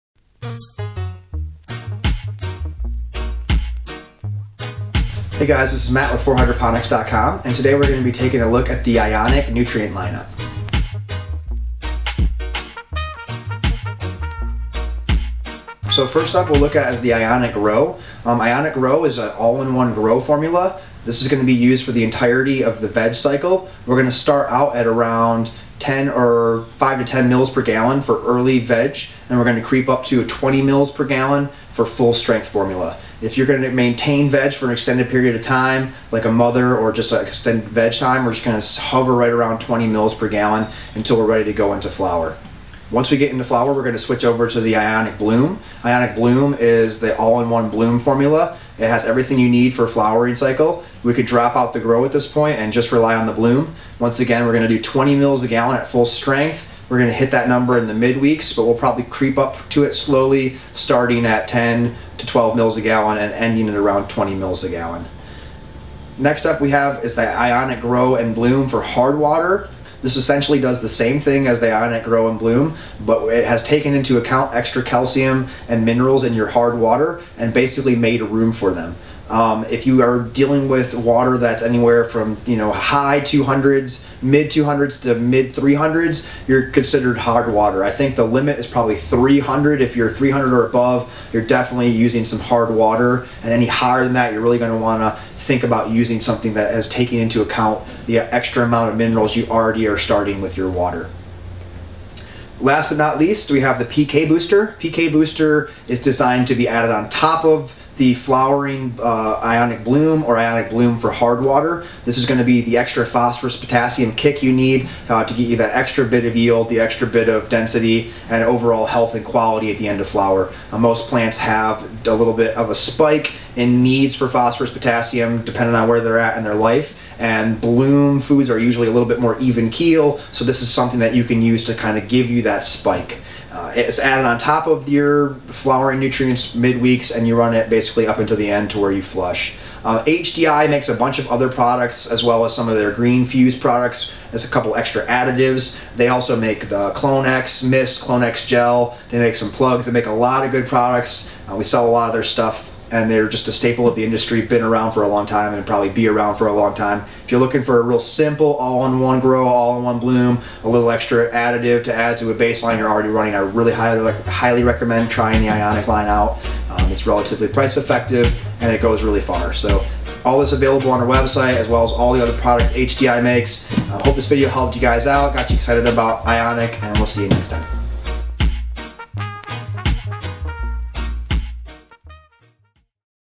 Ionic Nutrient Line Review
Ionic nutrients are formulated specifically for hydroponic cultivation. They are complete hydroponic nutrients containing all essential trace elements necessary for fruit and flowering. This family of hydroponics nutrients include Ionic Grow, Ionic Bloom and Ionic Boost, plus hardwater formulas for those who need them.

Ionic hydroponic nutrients are complete in every elemental detail and balanced for precise delivery of nutrients to the plant. Ionic's precision engineered nutrients are stable, balanced solutions and are easy-to-use. They provide highly stable pH due to unique reactive buffering incorporated into the manufacturing process. This reduces fluctuations in the tank and cuts down the workload for the grower.